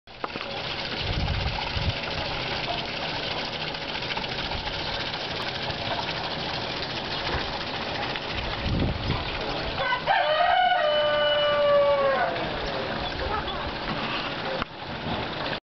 Waterwheel Stock Feeding System in Lancaster, PA
A pure mechanical installation takes advantage of the gravity such that the gravitational potential energy of water can be turned into kinetic energy.